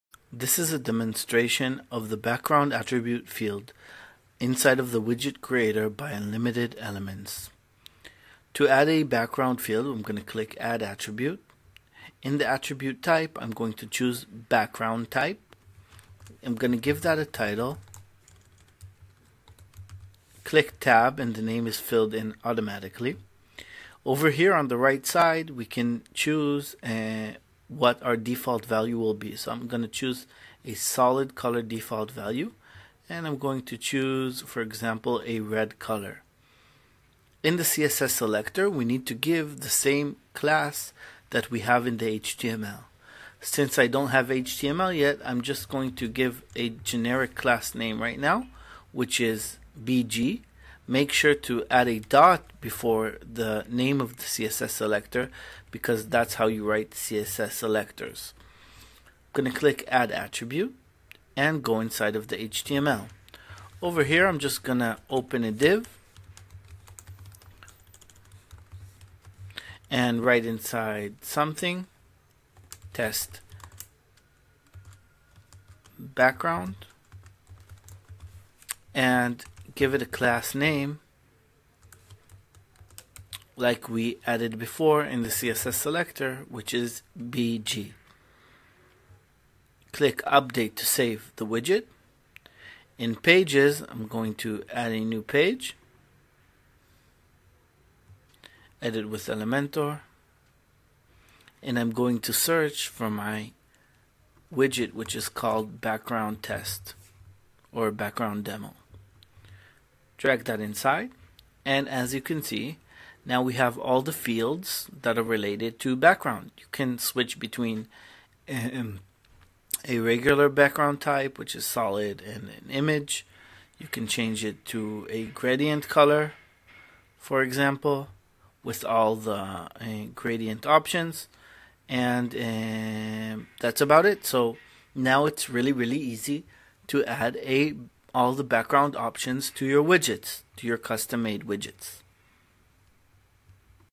Background Attribute - Unlimited Elements Widget Creator
In this video i show how to use the background field which outputs all of the css for background fields. Solid, Image, Gradient and even video. Now you can add editable backgrounds easily to your widgets.